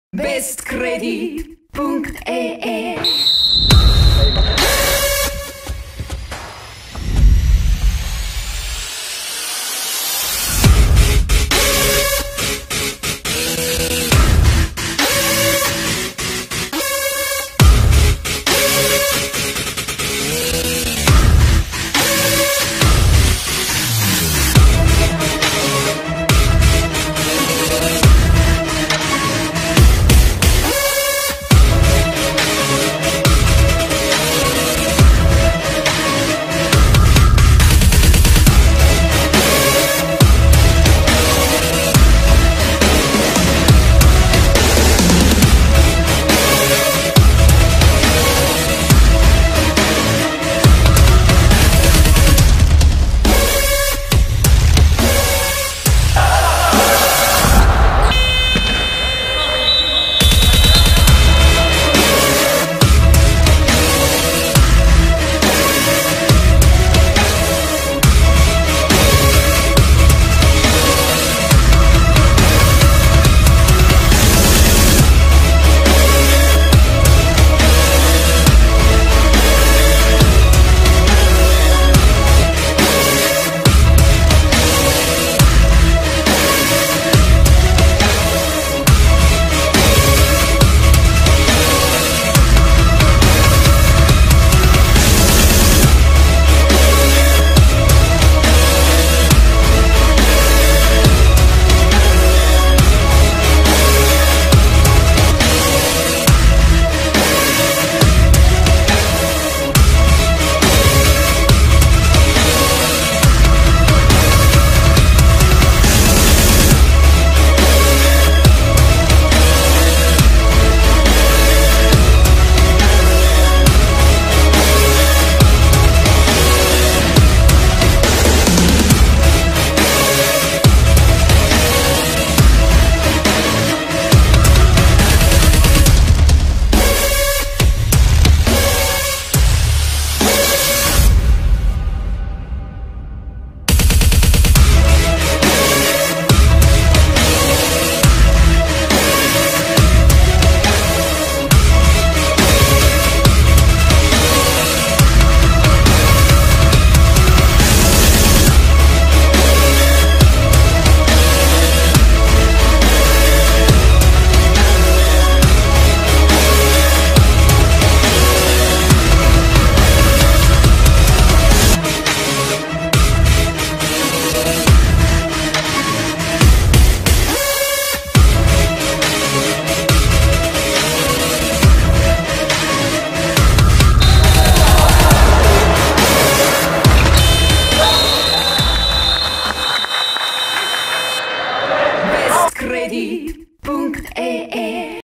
2014-12-20 Santeh - TÜ BestCredit 4:5 (Full HD)
Eesti Saalijalgpalli Meistrivõistlused 2014/2015. Esiliiga VIII voor.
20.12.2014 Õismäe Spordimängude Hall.

TÜ BestCredit väravad: Marek Naaris 2, Hindrek Teder, Alvar Kasak.

Filmitud kaameraga: Canon LEGRIA HF G10